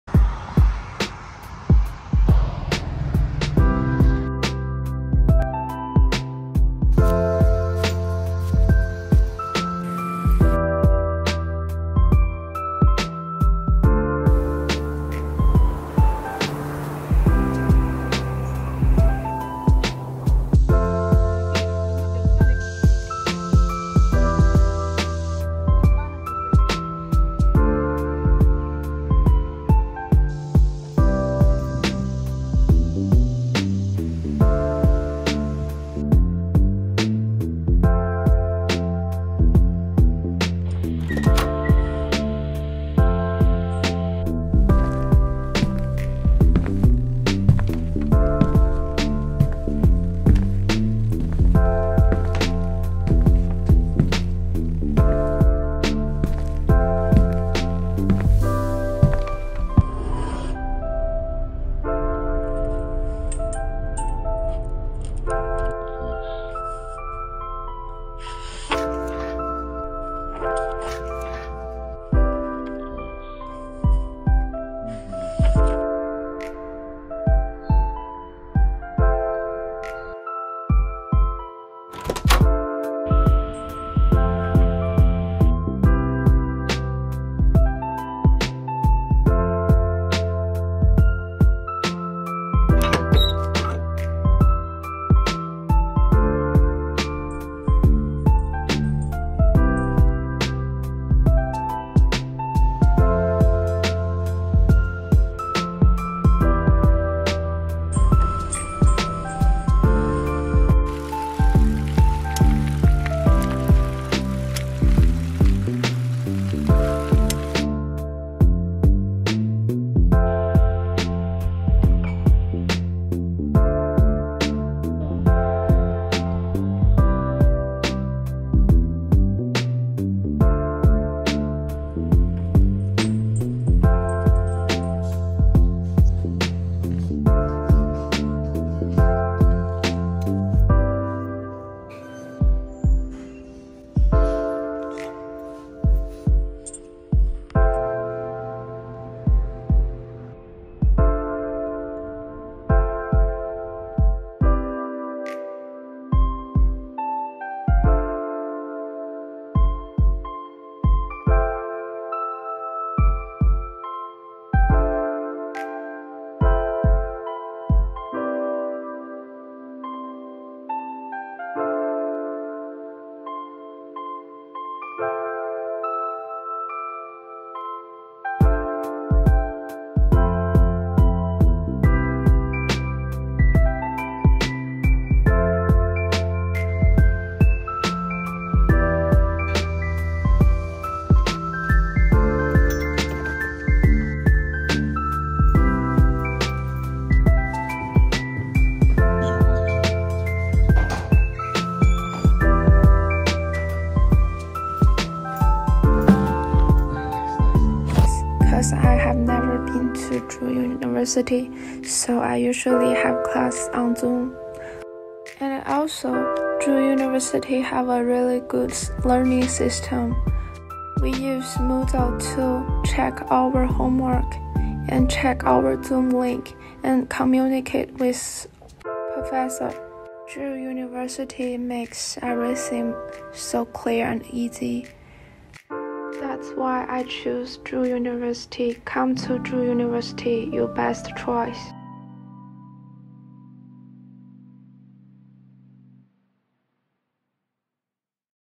A Day at Drew University
As a group working on class EAP202/EAP062/EAP520, we make this video to promote Drew University.